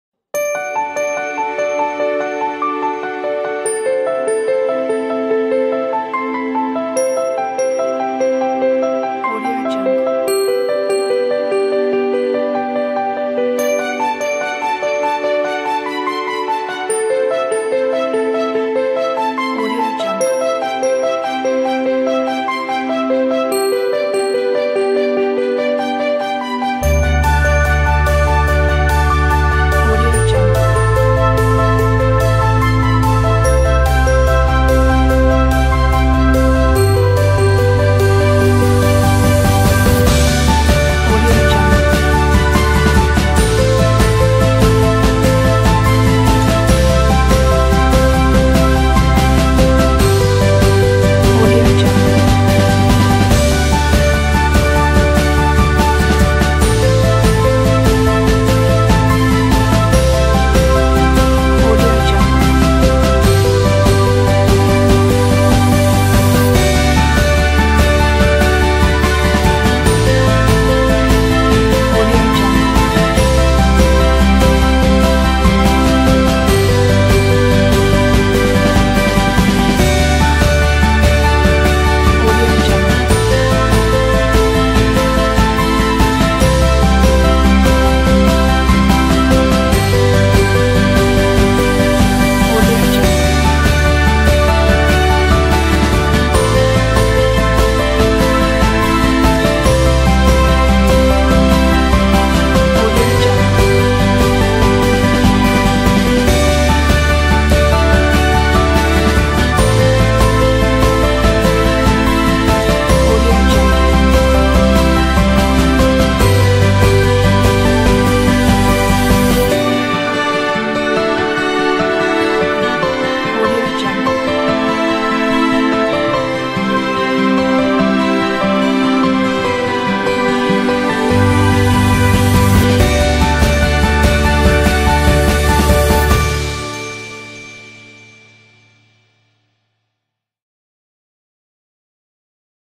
After Effects Project Files - Presentation of the Companys Employees - VideoHive 9965793
CONTENT: No plug-ins required Video tutorial included Resolution: Full HD 1920x1080 Frame rate: 25 - 29,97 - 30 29 photo or video holders CS5 38; Above Duration 02:22 Photos is not included:... (openers) Read more